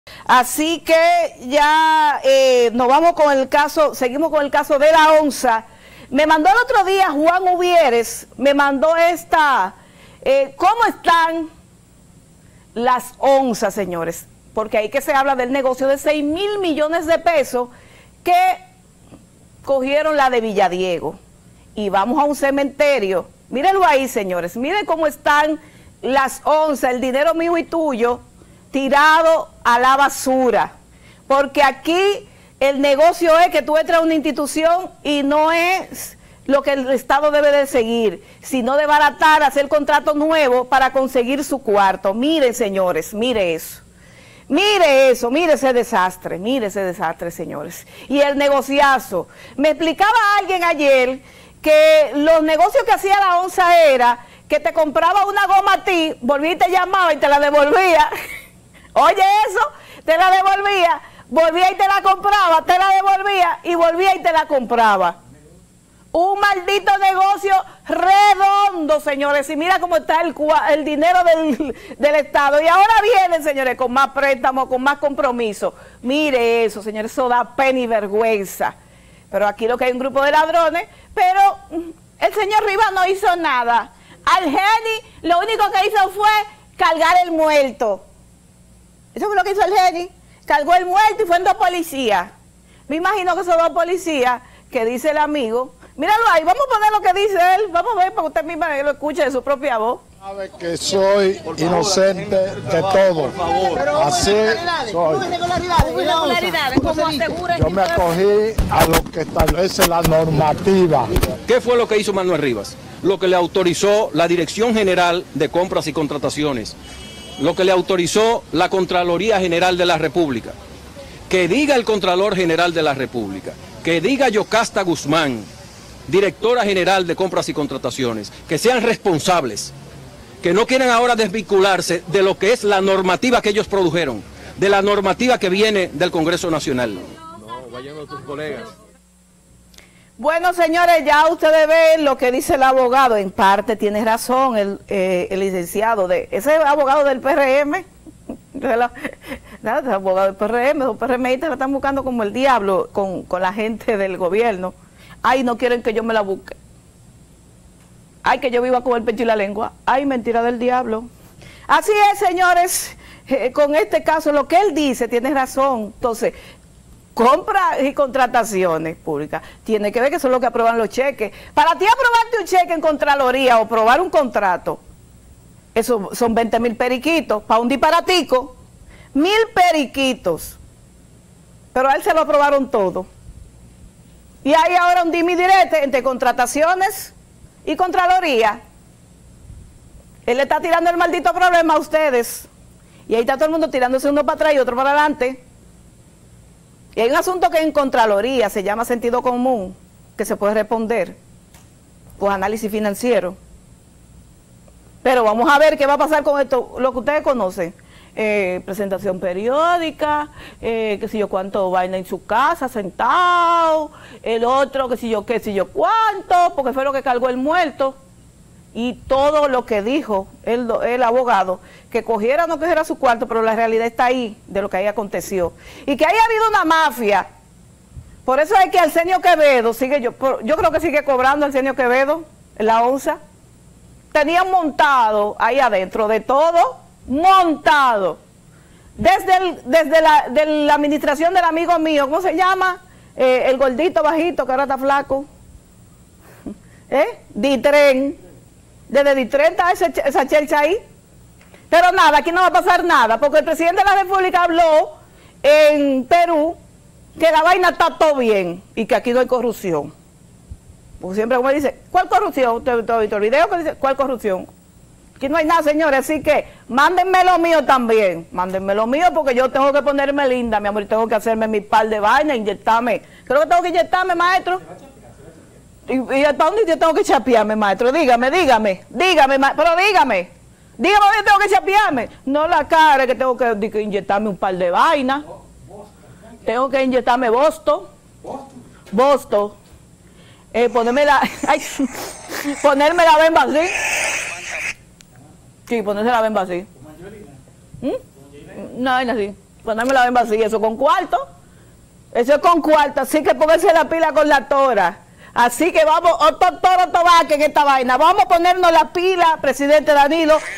La Tora denuncia y muestra imágenes de como es la corrupción en la OMSA
Canal oficial de la comunicadora más controversial de la actualidad en la República Dominicana.